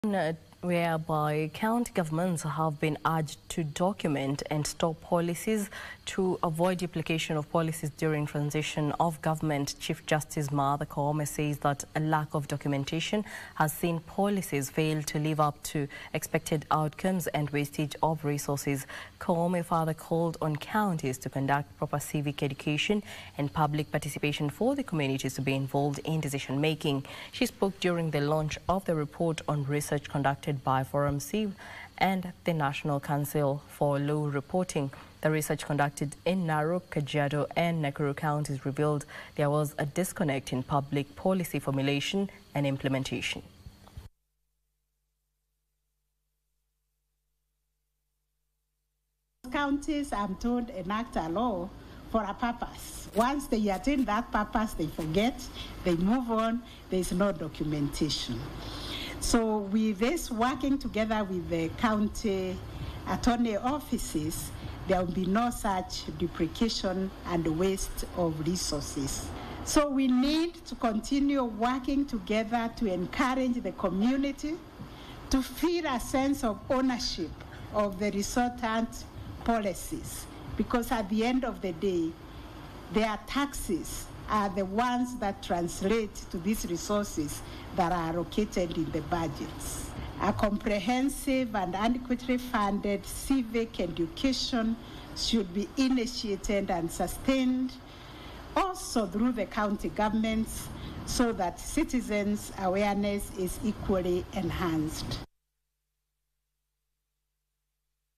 Counties urged to document policies
County governments have been urged to document and store policies to avoid duplication of policies during transition of government. Chief Justice Martha Koome says that lack of documentation has seen policies fail to live up to the expected outcomes and wastage of resources. Koome further called on Counties to conduct proper civic education and public participation for the communities to be involved in decision making.  She spoke during the launch of the report on research conducted by ForumCiv and the National Council for Law Reporting. The research conducted in Narok, Kajiado and Nakuru counties revealed there was a disconnect in public policy formulation and implementation.